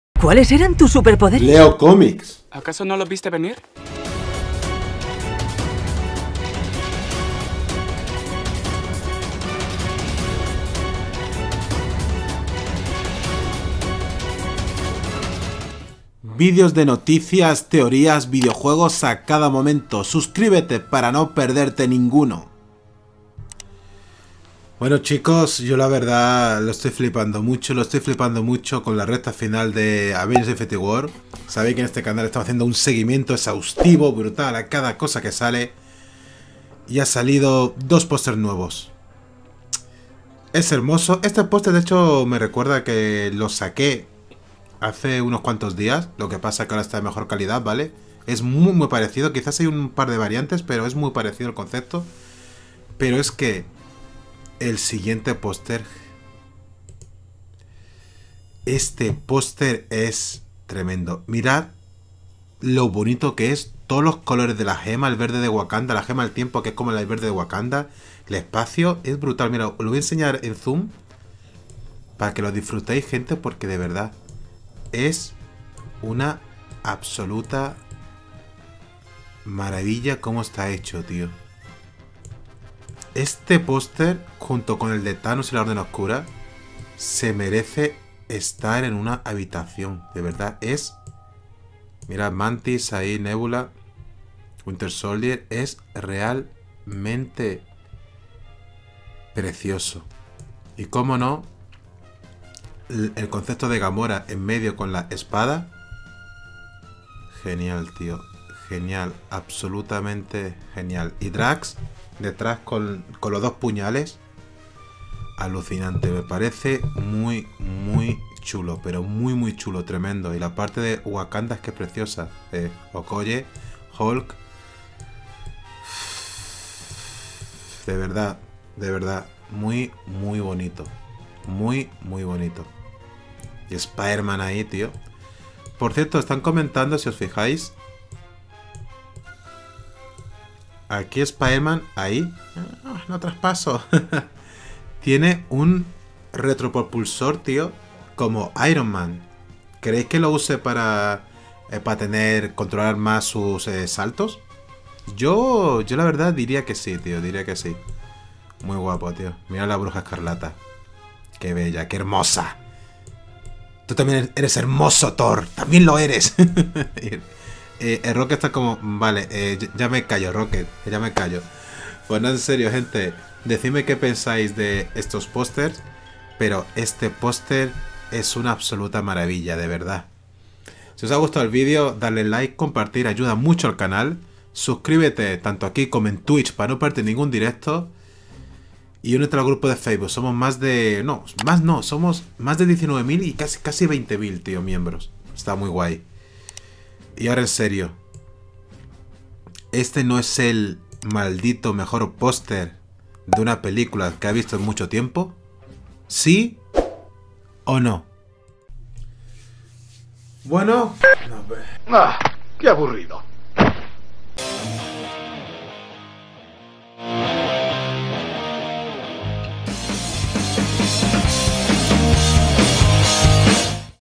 BRUTAL IRON SPIDER TIENE PROPULSORES COMO IRONMAN! Y POSTER NUEVO DE AVENGERS INFINITY WAR Y
💵APOYA POR YAPE CAUSA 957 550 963